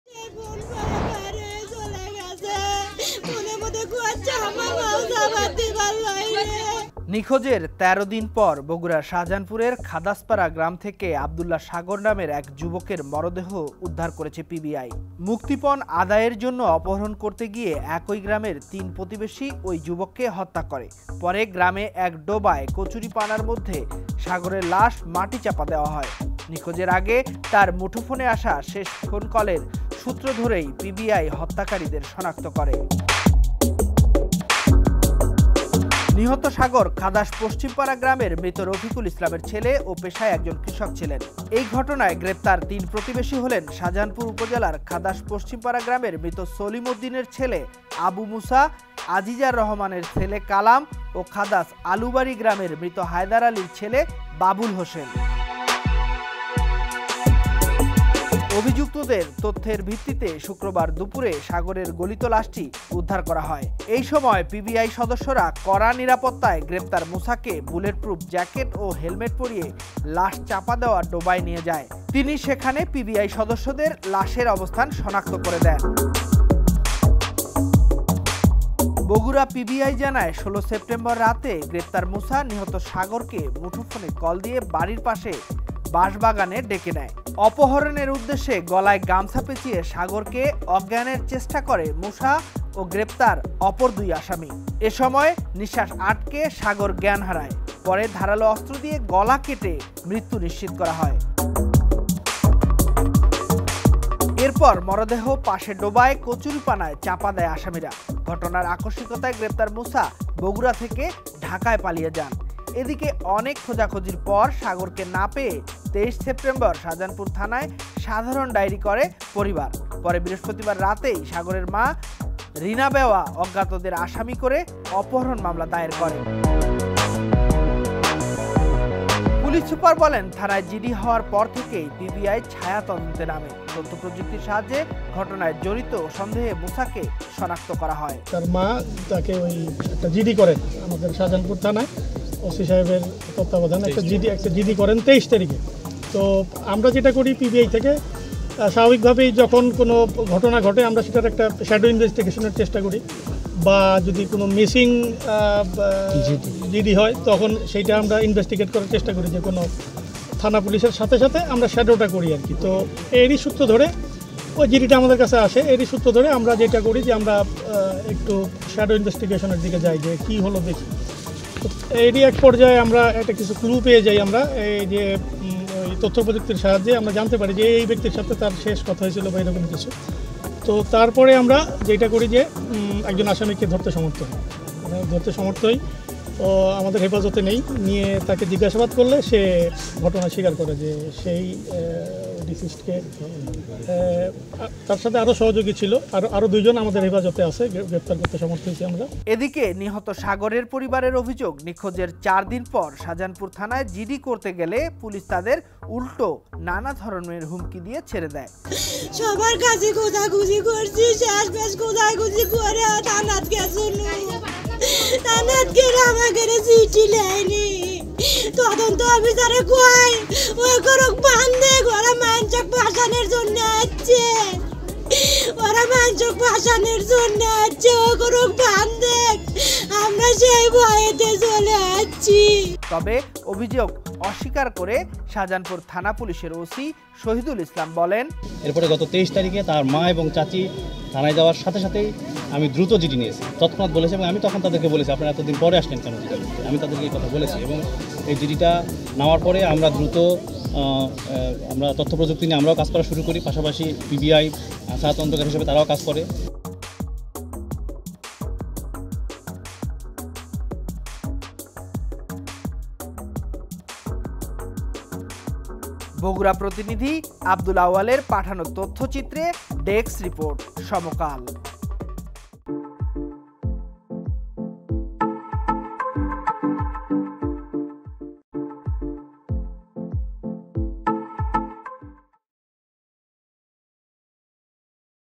নিখোঁজের ১৩ দিন পর কচুরিপানার ডোবায় পাওয়া গেল যুবকের ম/র/দে/হ | Bogra | Samakal News
নিখোঁজের ১৩ দিন পর কচুরিপানার ডোবায় আব্দুল্লাহ সাগর নামের এক যুবকের ম/র/দে/হ উদ্ধার করেছে পিবিআই। মুক্তিপণ আদায়ের জন্য অপহরণ করতে গিয়ে একই গ্রামের তিন প্রতিবেশি তাকে  হ/ত্যা করে। নিখোঁজের আগে তার মুঠোফোনে আসা শেষ ফোনকলের সূত্র ধরে পিবিআই হ/ত্যাকারীদের শনাক্ত করে...